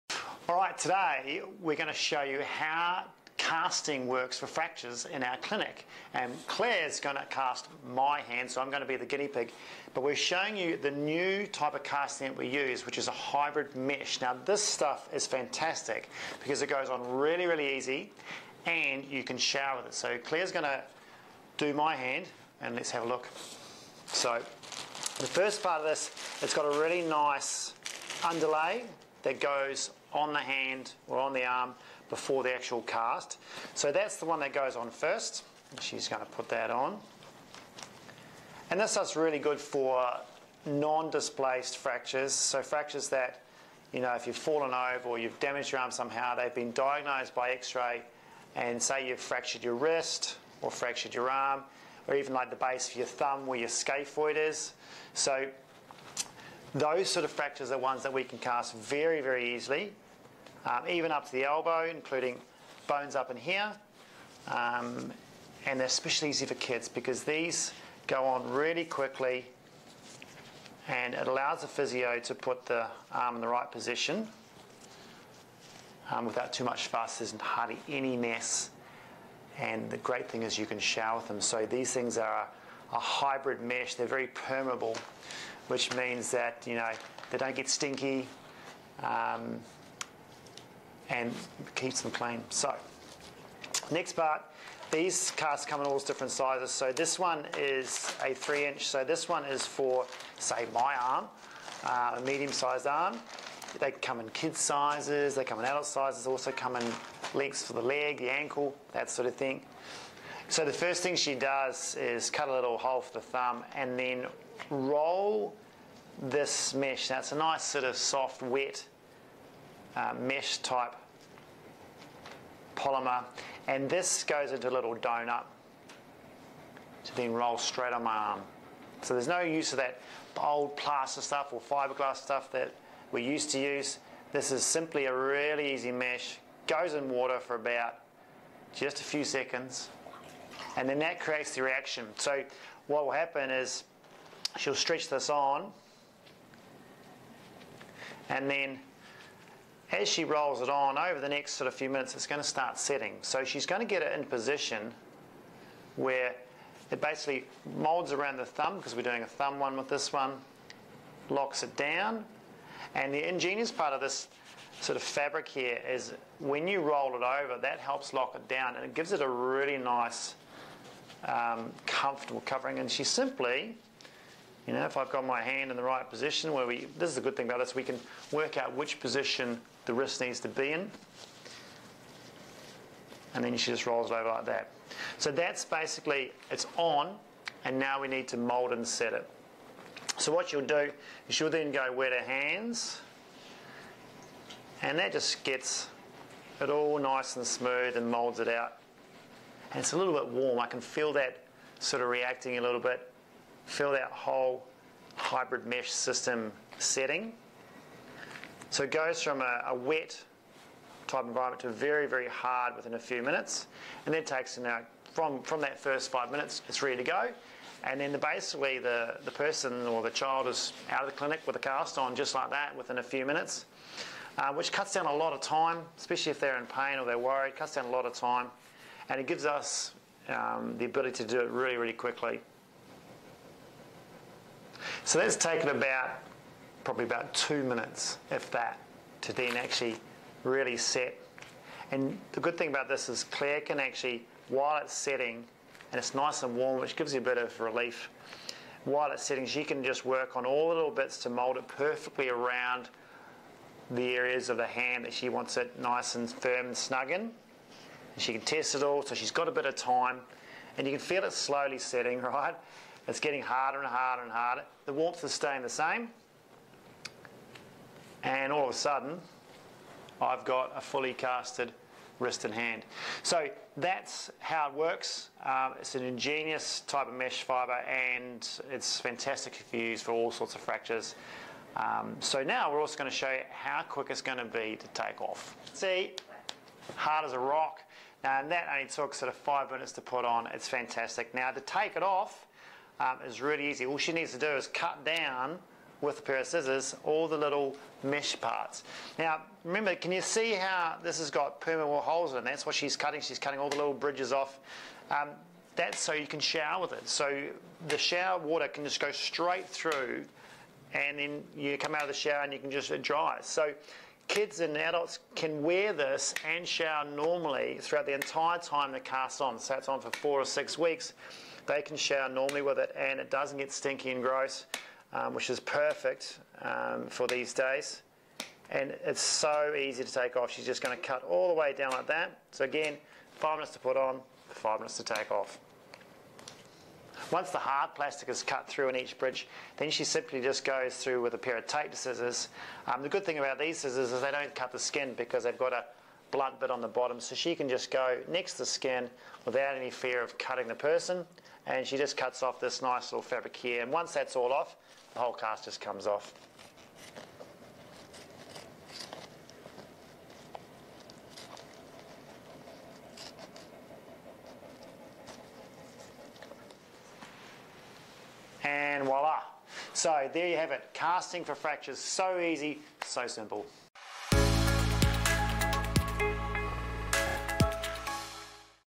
Casting of Fractures using Hybrid Mesh | Tim Keeley | Physio REHAB
In our clinic we use a hybrid mesh cast for fractures (broken bones) which is so simple and easy to use, its not messy - AND you can shower with it!1 No more stinky casts or using plastic bags in the shower.

The physio can cast the fracture right in the clinic - no need to wait at hospital emergency department, you can just come straight in after your X-Ray as it takes literally less than 5 minutes to put on and less than 5 minutes to take off. Great for simple fractures of the wrist, hand, forearm, elbow and ankle.